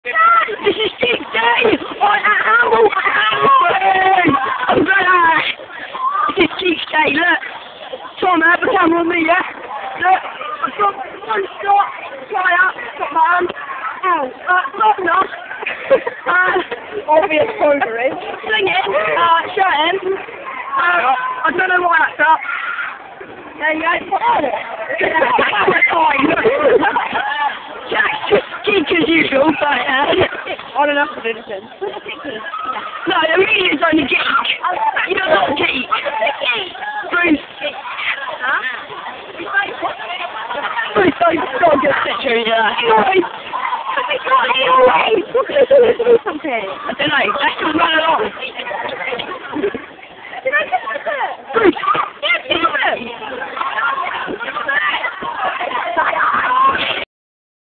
me,tom,abi and emilia being geeks!
lunch time of 9/9/09,we were all geeks